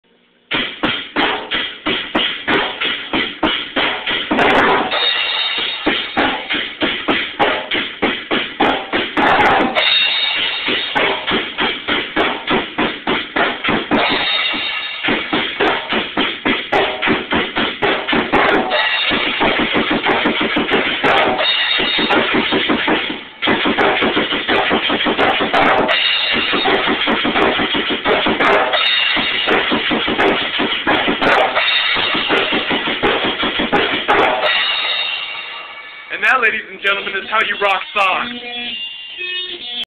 Me on my First Day of Drums!!
sorry for the Crappy quality..shot on phone..